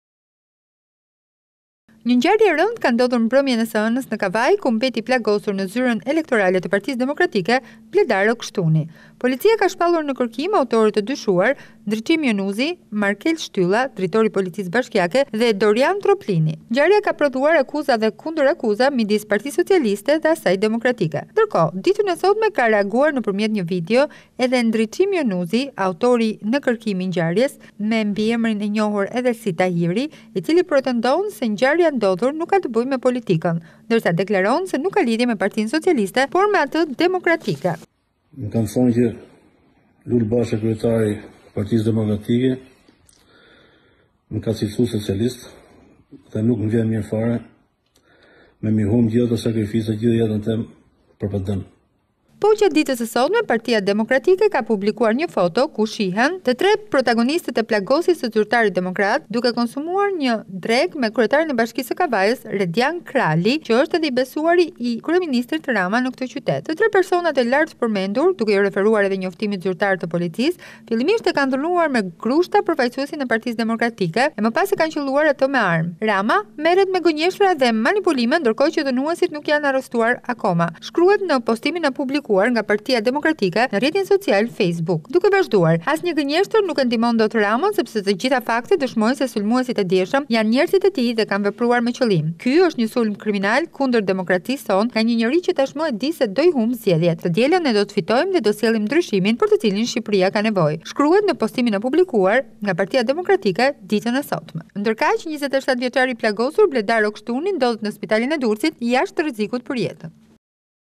Plagosja e aktivistit demokrat brenda zyrës elektorale shndërrohet në mollë sherri midis palëve
Durrës Plagosja e aktivistit demokrat brenda zyrës elektorale shndërrohet në mollë sherri midis palëve